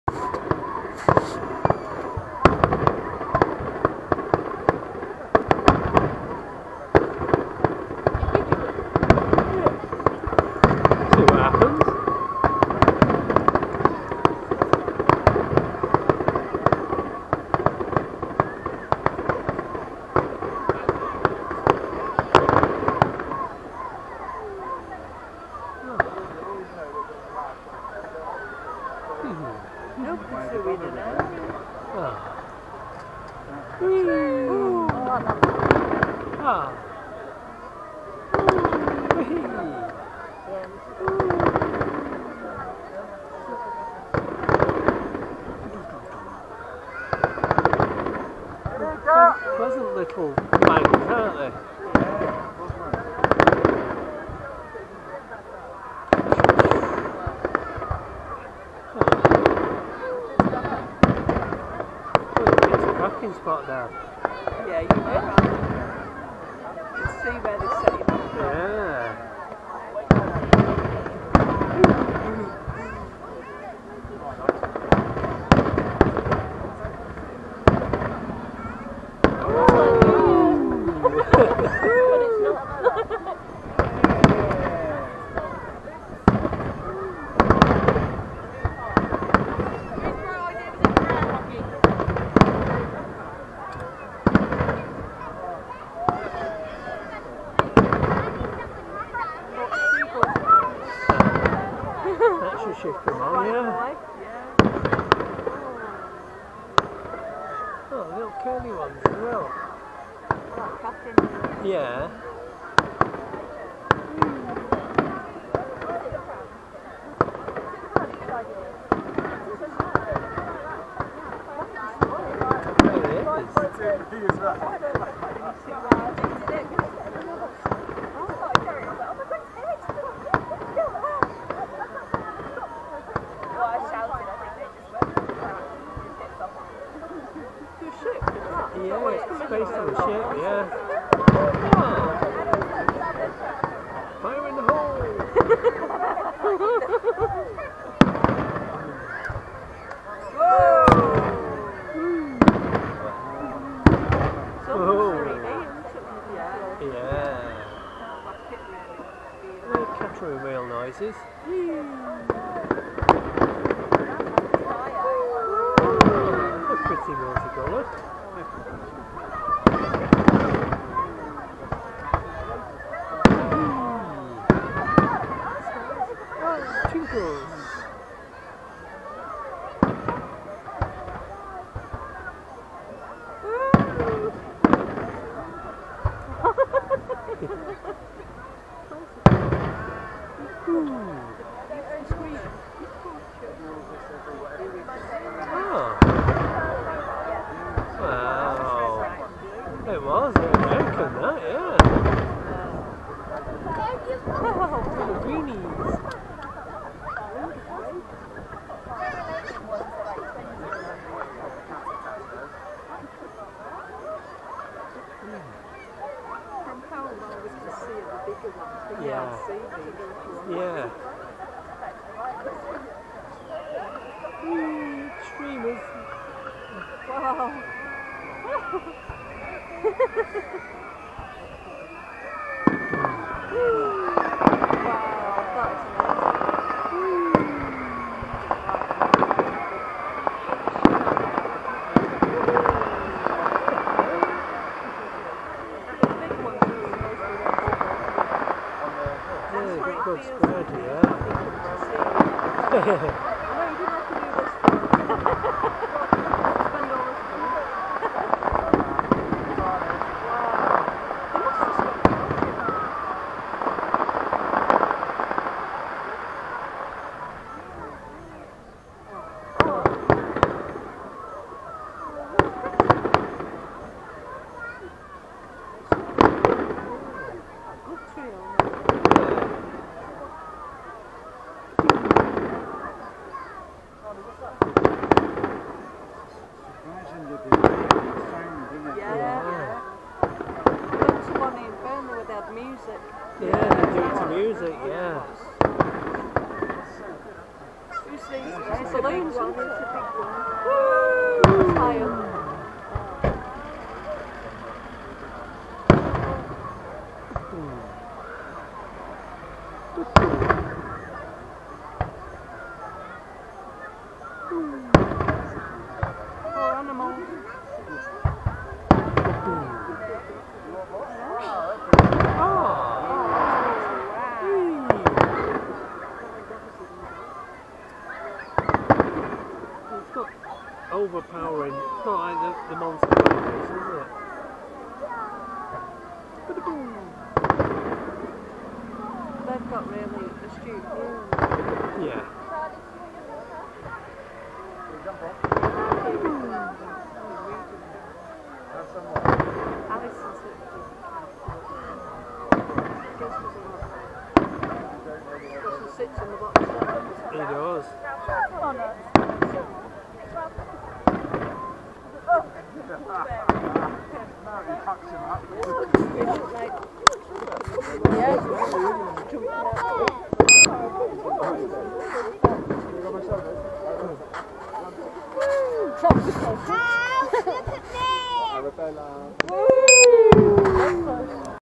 Weymouth fireworks 3D
3D side by side view of fireworks from SID camera.